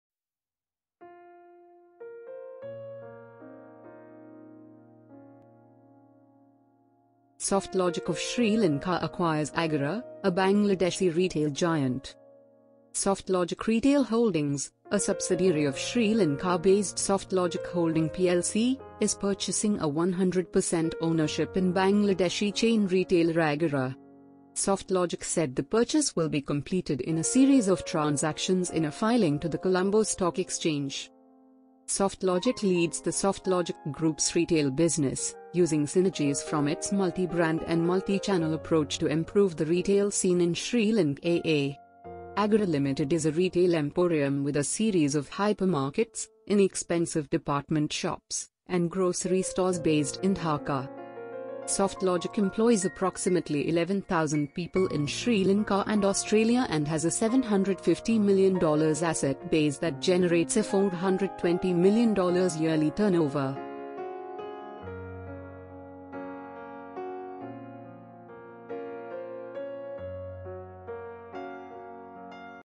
Softlogic of Sri Lanka acquires Agora, a Bangladeshi retail giant | NewsRme
Softlogic Retail Holdings, a subsidiary of Sri Lanka-based Softlogic Holding Plc, is purchasing a 100 percent ownership in Bangladeshi chain retailer Agora.
Softlogic said the purchase will be completed in a series of transactions in a filing to the Colombo Stock Exchange.

Softlogic leads the Softlogic Group's retail business, using synergies from its multi-brand and multi-channel approach to improve the retail scene in Sri Lanka.
Agora Ltd is a retail emporium with a series of hypermarkets, inexpensive department shops, and grocery stores based in Dhaka.

Softlogic employs approximately 11,000 people in Sri Lanka and Australia and has a $750 million asset base that generates a $420 million yearly turnover.